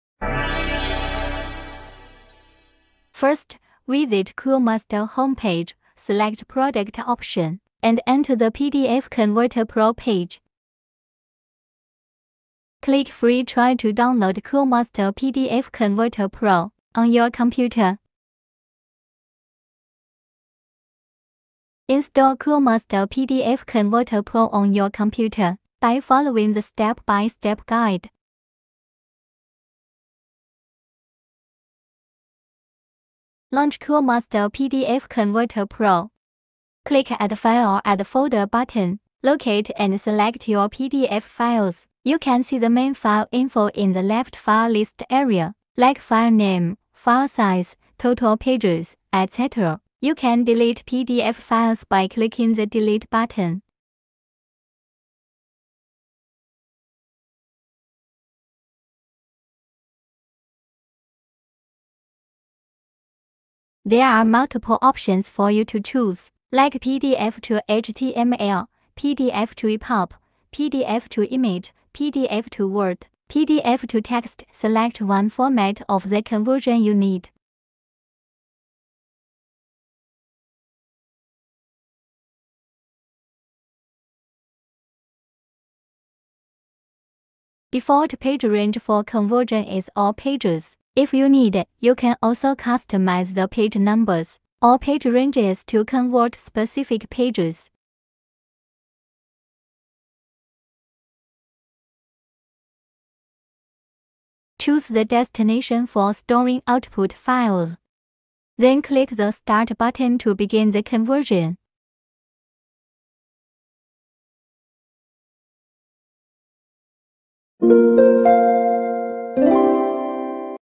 Coolmuster PDF Converter Pro: Convert PDF to Other File Formats
Coolmuster PDF Converter Pro is an all-in-one desktop application, which is dedicated to helping all Windows users to convert PDF files to Word, ePub, Text, Images, HTML, etc. easily.

Key Features:
• Export Adobe PDF to Word, ePub, Text, Images, HTML, etc.
• Batch convert a 200-page PDF within 1 minute, super fast.
• Built-in preview window enables you to preview and set page ranges before converting.
• You can customize and define the page range or page ranges for partial converting.
• Preserve the original text contents, layouts, formatting, hyperlinks, images, etc. in the output files without any quality loss.